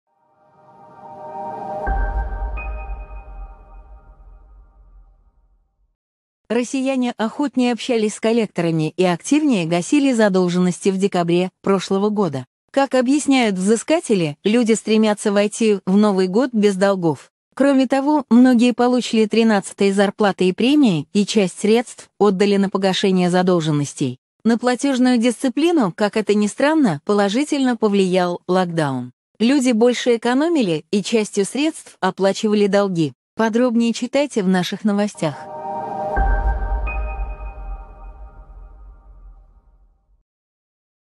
К концу года россияне охотнее гасили задолженности
Как отмечают коллекторы, должники охотнее шли на контакт и чаще сдерживали обещание погасить долг в декабре прошлого года. Подробнее